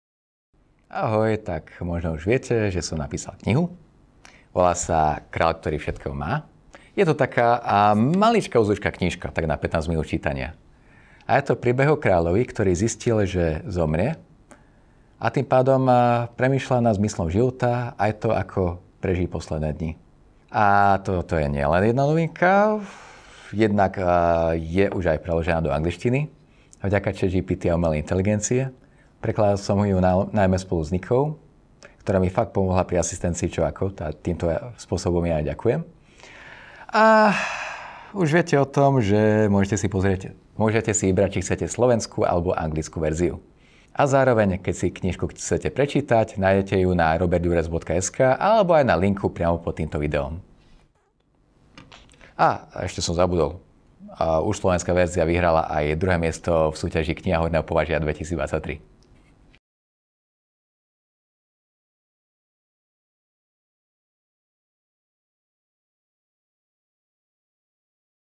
KRÁĽ, ktorý všetkého MÁ
Kráľ, ktorý všetkého má, je príbeh o kráľovi, ktorý mal všetko hmotné, ale na niečo zabudol. Zabudol žiť. Uvedomil si to až v okamihu, keď zomiera. Vydáva sa na cestu a hľadá zmysel svojho života. V knihe nájdete mnoho myšlienok a postrehov, ktoré pri svojom hľadaní objavil. Táto malá útla knižka je ideálna pre dospelých - je iba na 15 minút čítania.

Kniha vyhrala 2. miesto v súťaži Kniha Horného Považia 2023 v kategórii Beletria. Kniha je dostupná aj bezplatne online.

Zaujímavosť na knihe: Anglická verzia bola preložená pomocou Chat GPT, a môžete si porovnať slovenský originál aj anglický preklad.

Autor knihy:
Róbert Ďurec